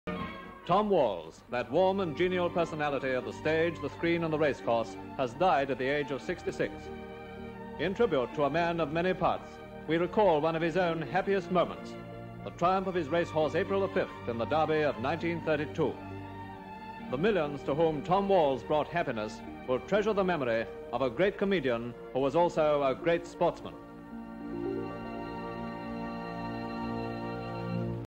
Tom Walls dies at 66 (1949)
Actor, comedian and horse racing enhusiast Tom Walls, whose horse April the Fifth won the Epson Derby in 1932, dies at age 66

Full Description:

ENGLAND:
EXT

actors, personalities, tribute, tributes, memorial, obituary, celebrity, celebrities
Background: Actor, comedian and horse racing enhusiast Tom Walls, whose horse April the Fifth won the Epson Derby in 1932, dies at age 66